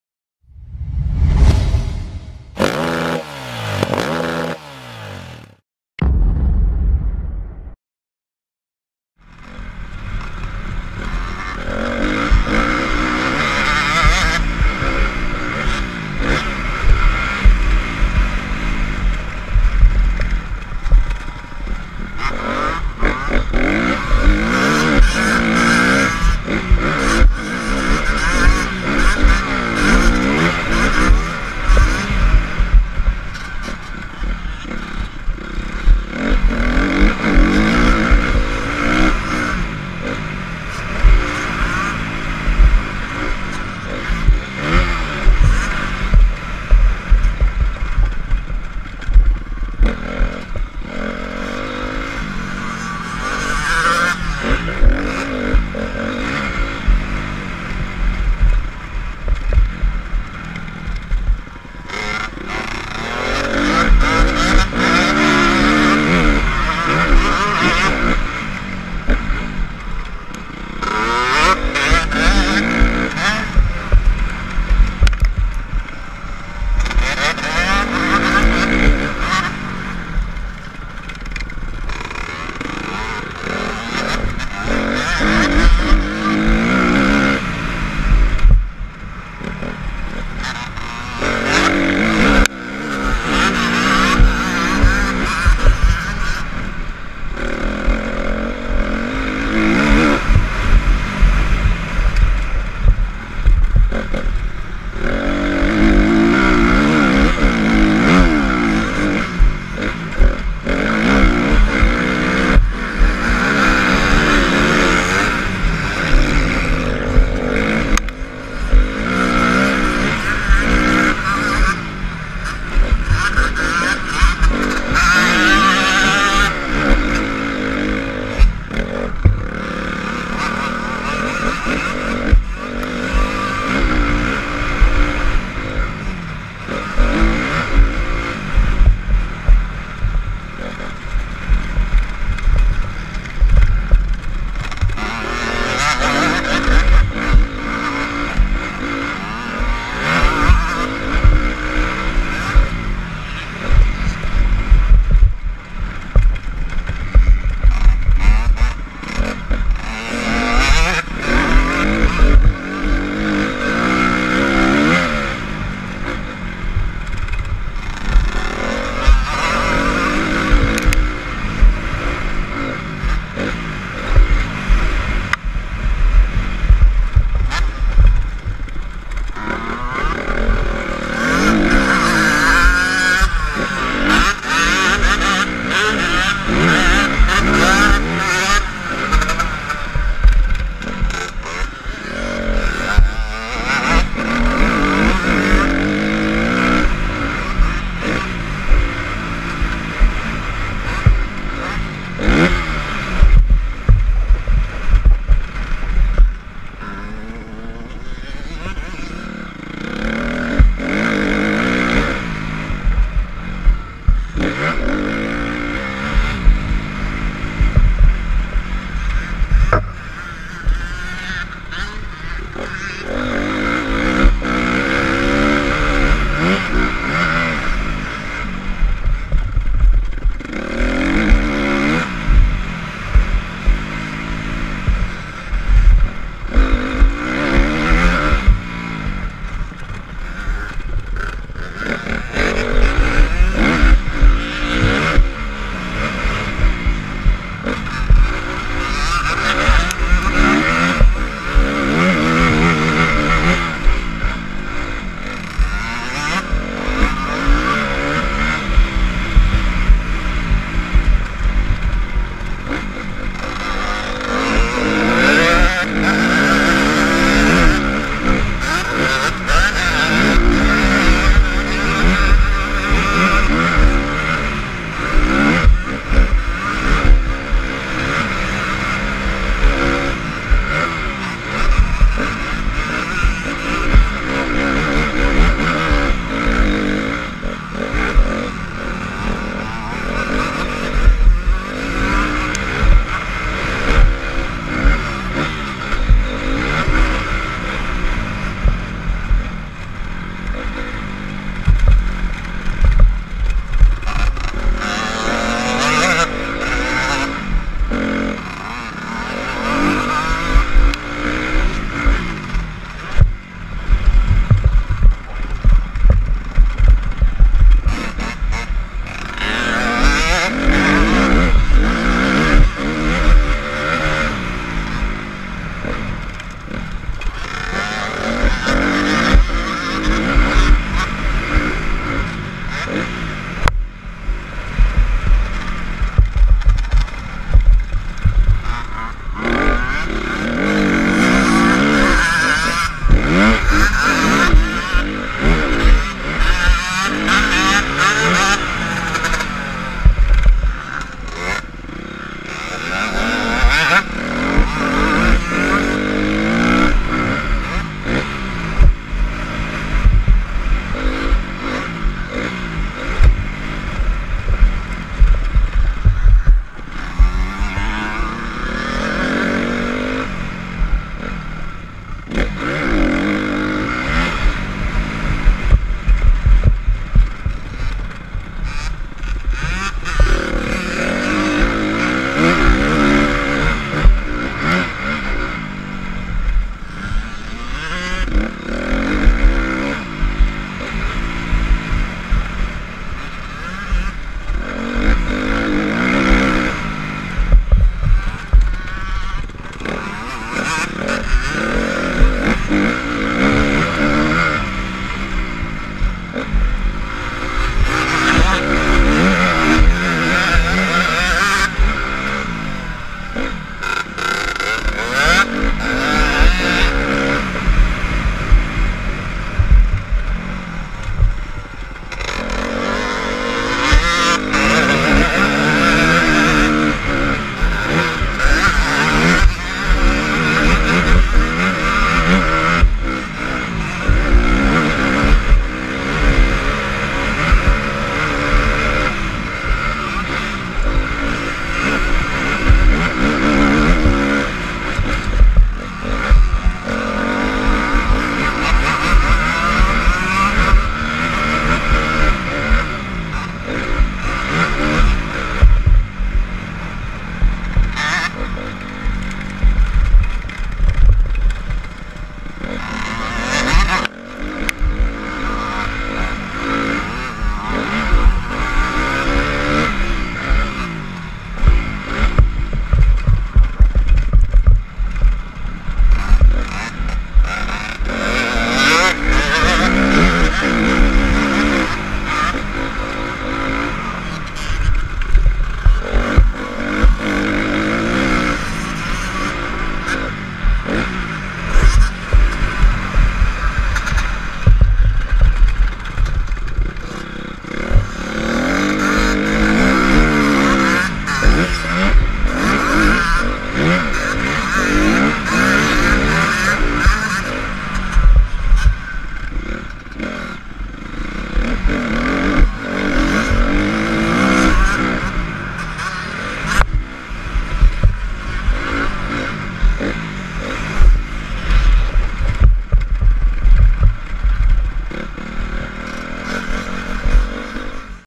GoPro HD: Honda CRF250 VS Yamaha YZ125 - Castellarano MX
Qualche giro di pista con Matteo Orsi